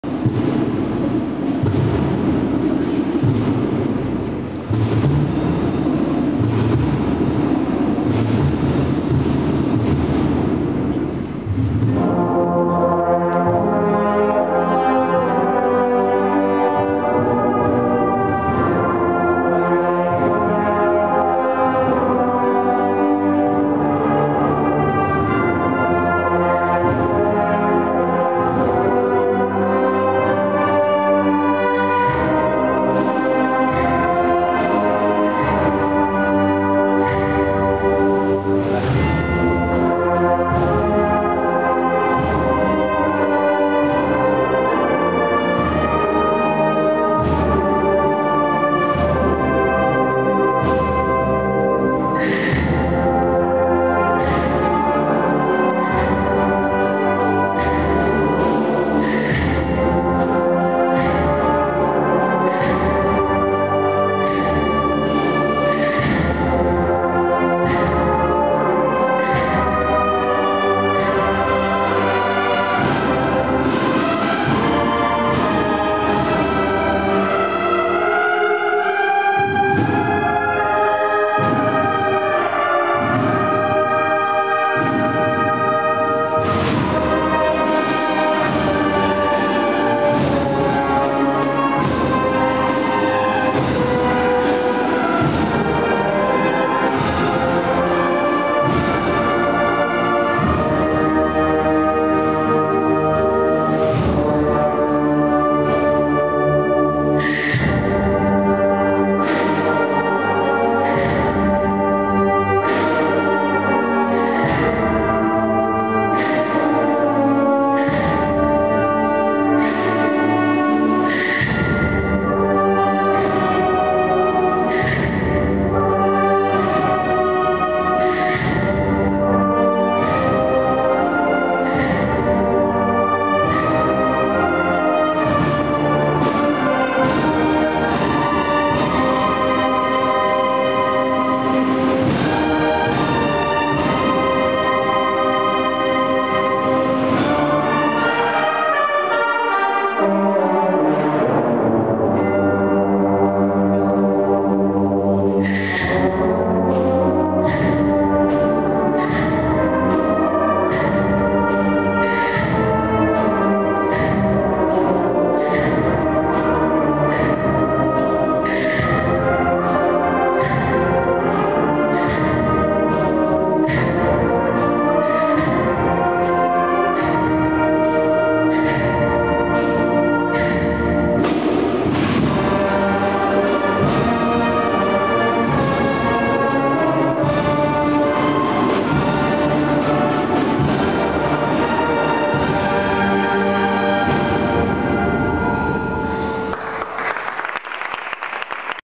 Llegada de la AM Cristo Yacente (Salamanca) al Santuario de los Gitanos 2015
Llegada de la AM Cristo Yacente de Salamanca, al Santuario de la Hdad de los Gitanos, en la noche del 28 de Noviembre del 2015